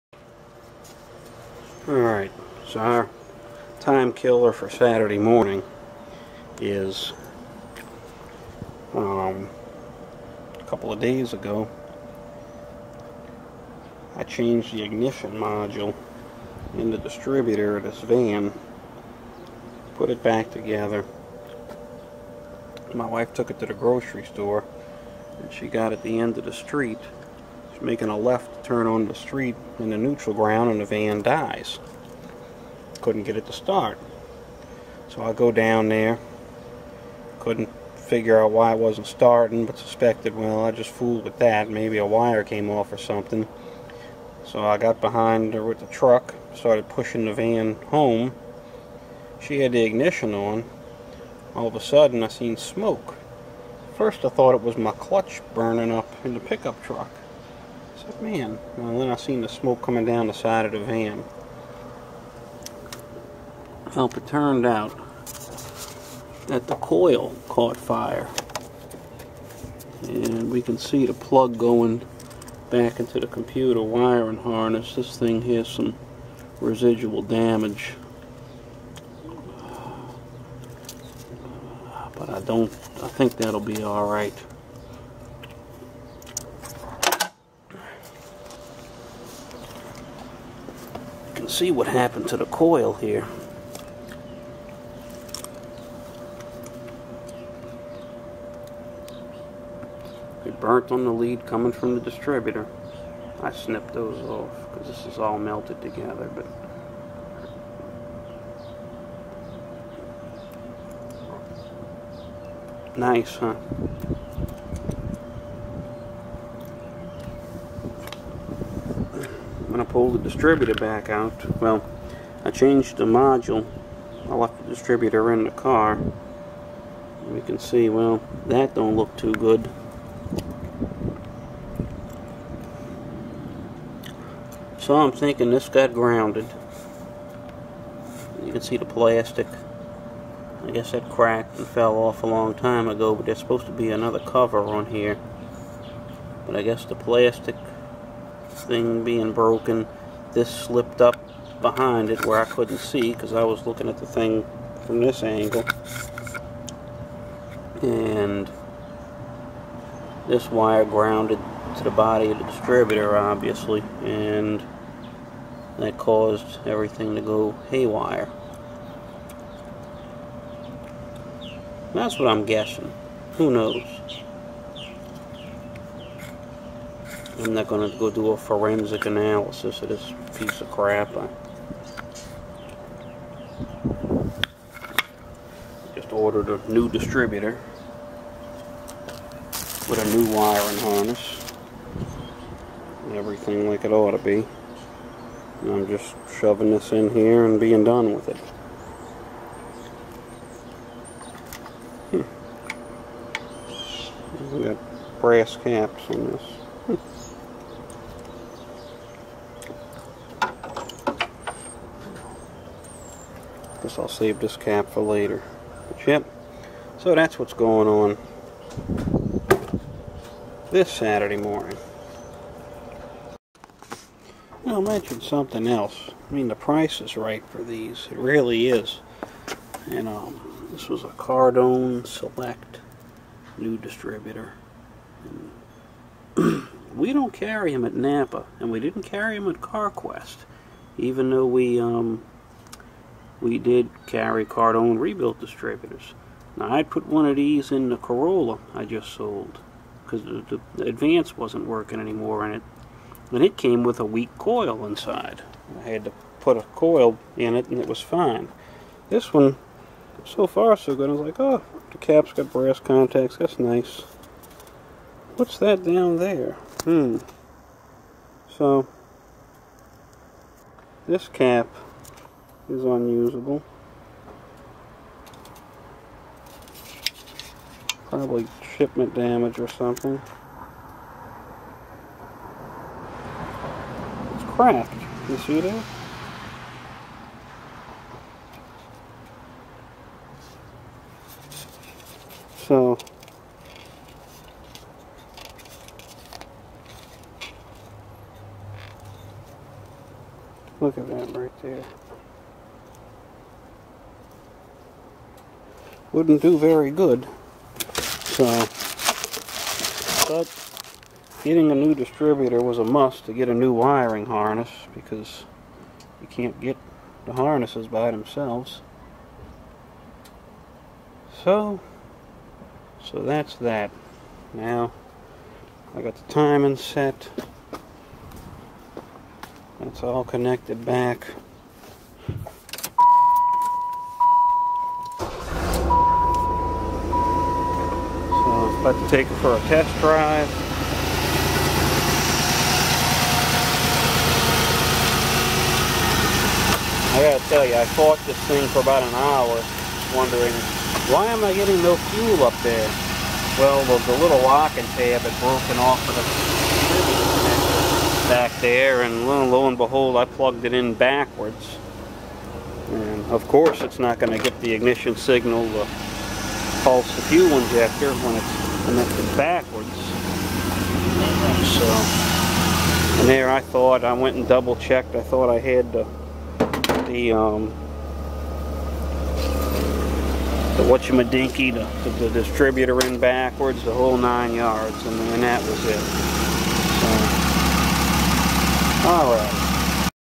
1995 Chevy Lumina Van - Fire!
Short in distributor lit up the ignition coil.